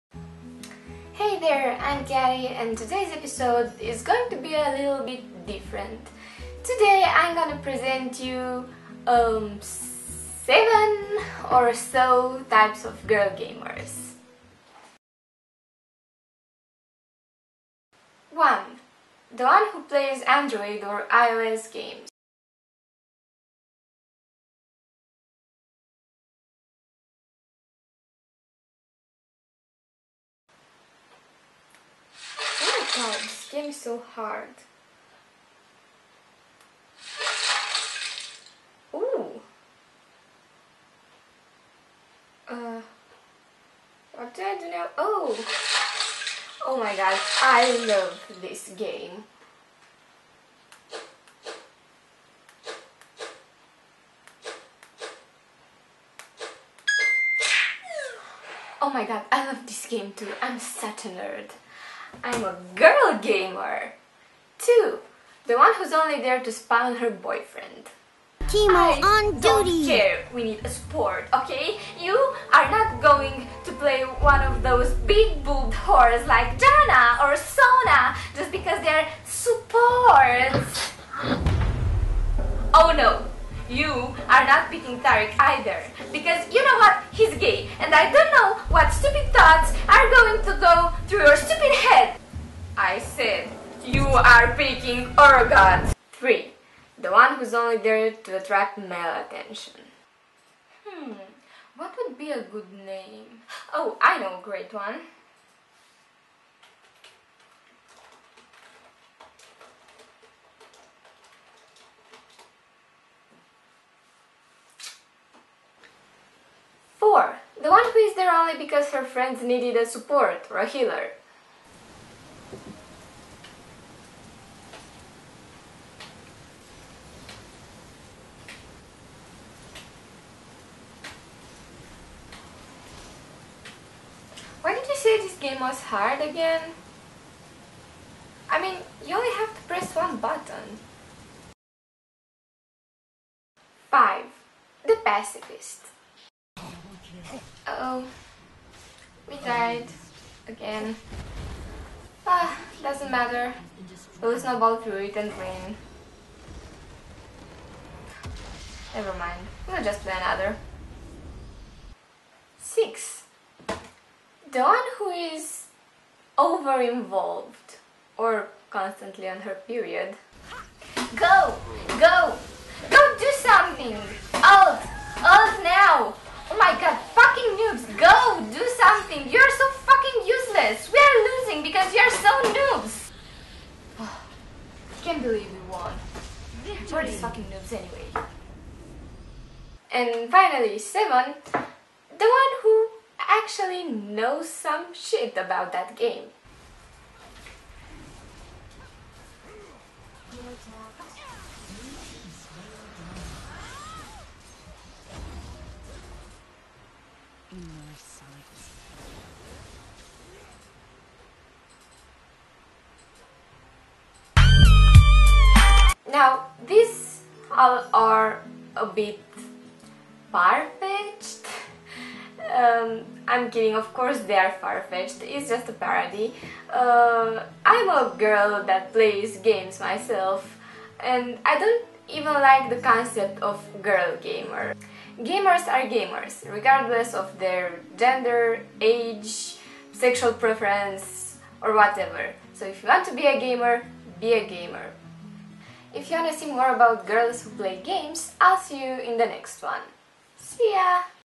7 Types of "Girl Gamers"
In this video I present some of the types, stereotypes and forms of girls who are either gamers or wannabes. However, this doesn't mean that there are no boys doing some of these things. Yes, I am watching you!